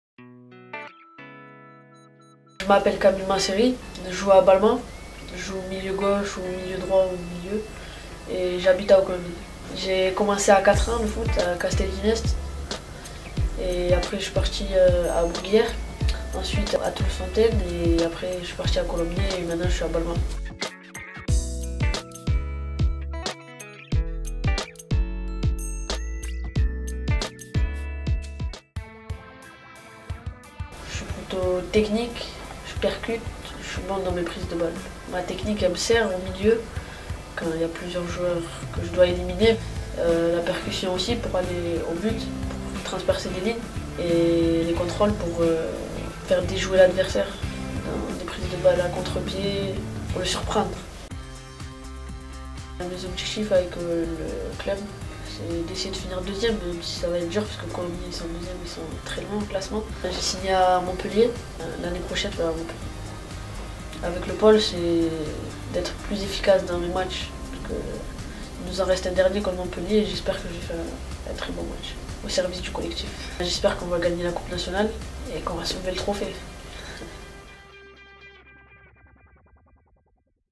Kamil MANSERI - Pôle Espoir Castelmaurou - Promo 2001
Découvrez Kamil MANSERI, un milieu de terrain technique qui évolue au SC Balma, et également au Pôle Espoir de Castelmaurou.